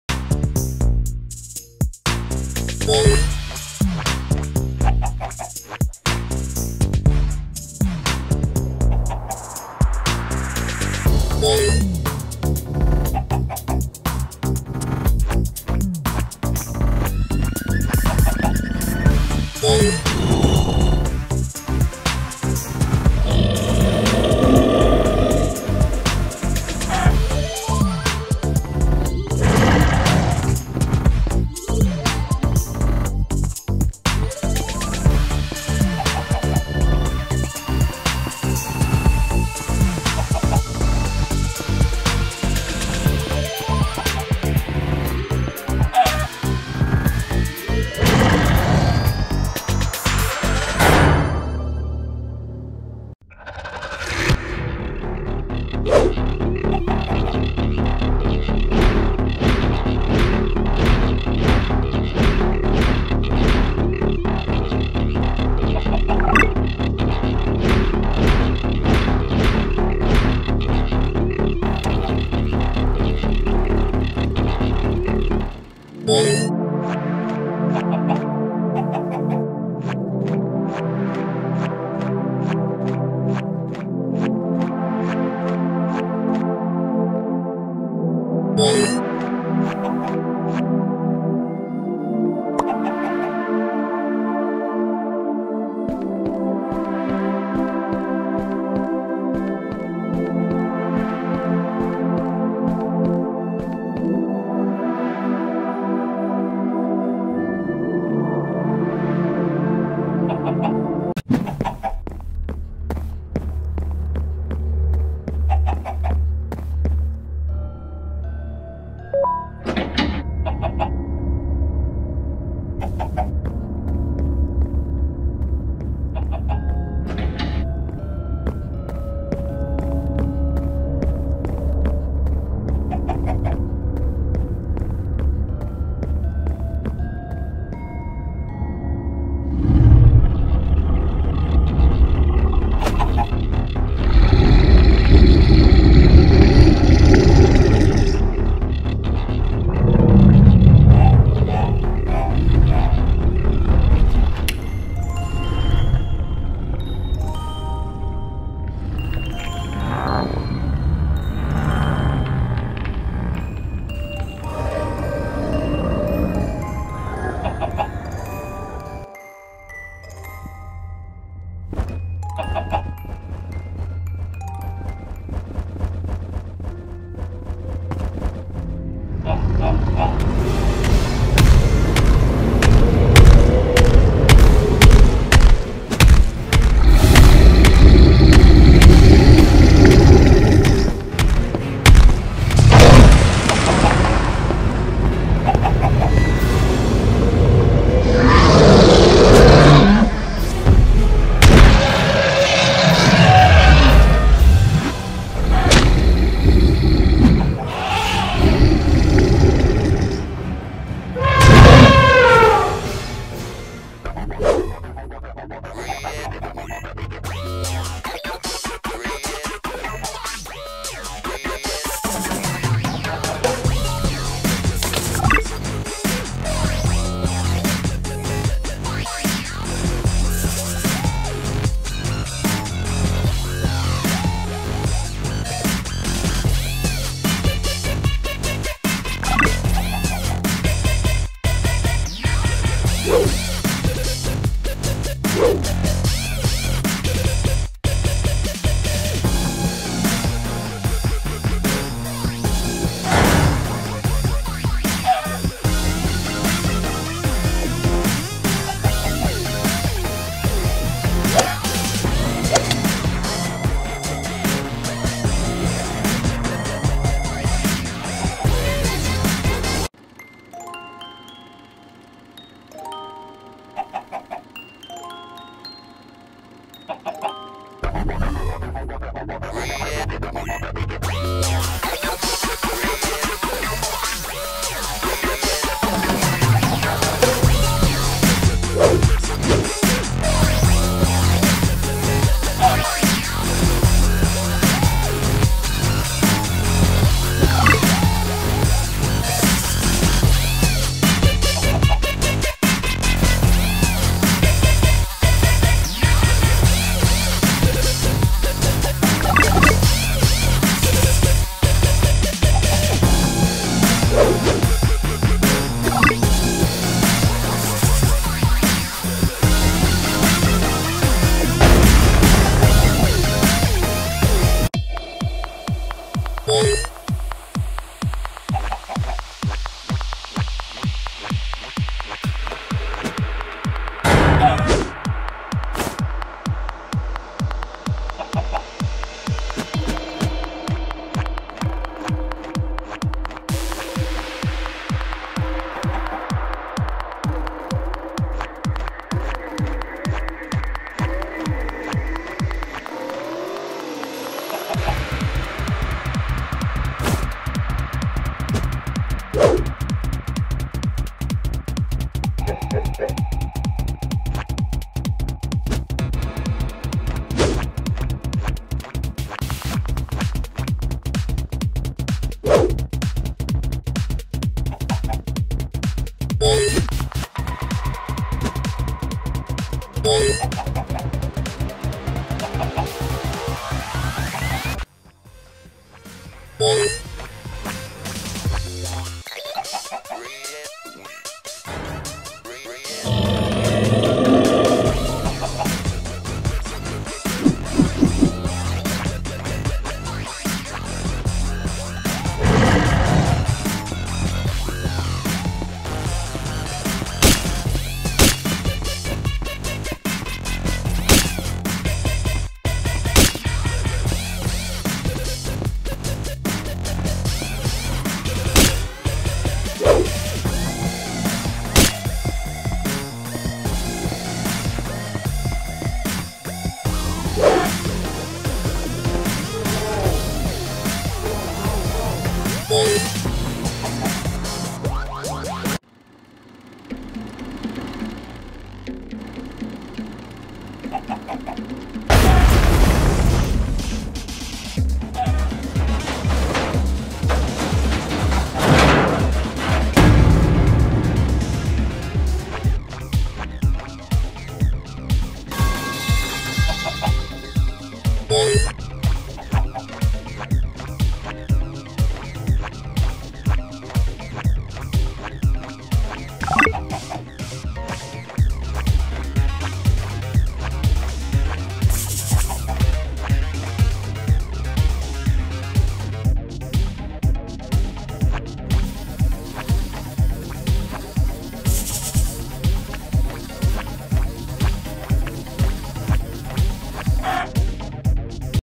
OPILA BIRD and COACH PICKLES vs EVIL BANBAN in GARTEN of BANBAN Chapter 3 ?! (Minecraft Animation)
COACH PICKLES Meeting with EVIL BANBAN in GARTEN of BANBAN 3 ?! (Minecraft Animation)

Noway!COACH PICKLES Meeting with EVIL BANBAN in his secrets rooms,but whats next?

CHARACTERS IDEA:  @buggyhuggy

This is a Minecraft Animation in the game Garten of Banban Chapter 3 !